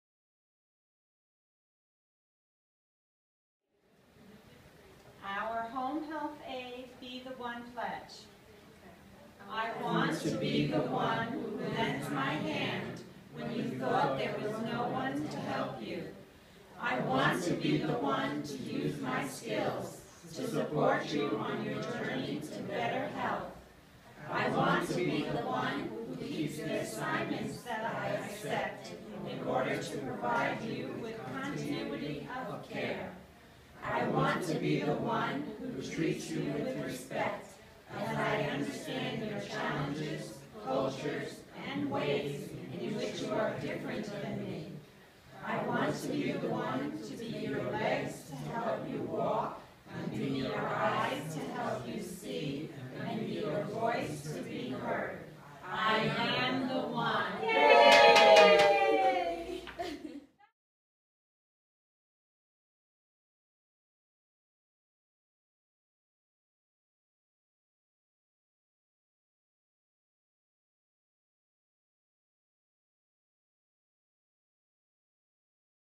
Selfhelp Community Services Home Health Aide Training
The "Be The One" pledge recited at the end of the Selfhelp Home Health Aide Training Program